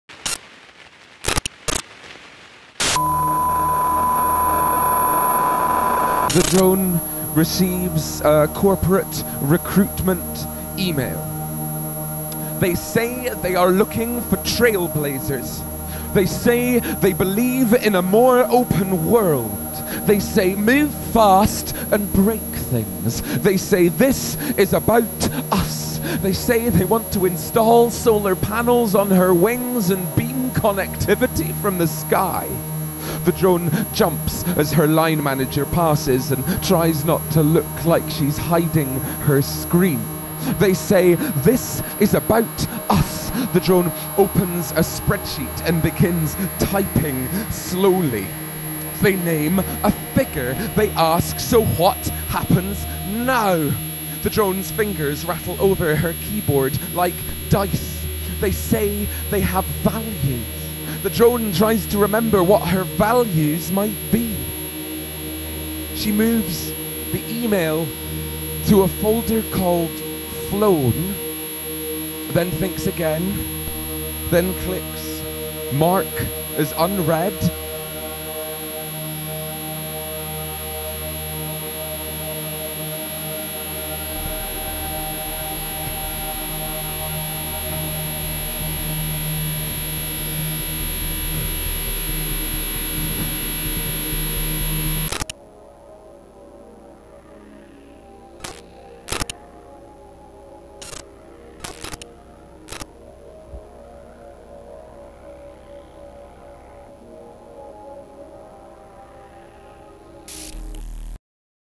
Drone Tour 2019: The Drone Receives a Corporate Recruitment Email
9 May: Sound Archive, Kirkwall: Tickets Coming Soon
10 May: Gable End, Hoy: Tickets Coming Soon

Drone is a live jam of sound, visuals and poetry, telling the story of a military drone’s life and fears. The Drone is part weapons system, part office worker, part tense background hum. The bleak humour and tender fury of Drone sees the unmanned aerial vehicle as the technology of a neurotic century, surveilled and surveilling, asking how anxious people can live as part of systems of such astonishing destruction.

Written by the Forward-shortlisted poet Harry Josephine Giles and performed by them with international sound artist Neil Simpson (Buffalo buffalo buffalo Buffalo buffalo) and digital artist Jamie Wardrop (Beats, The Dwelling Place), Drone is the spectacular culimination of five years of work. Directed by Rob Jones, the performance is mixed new every night, the three artists making a live cabaret band for an intense and groundbreaking multimedia poetry show.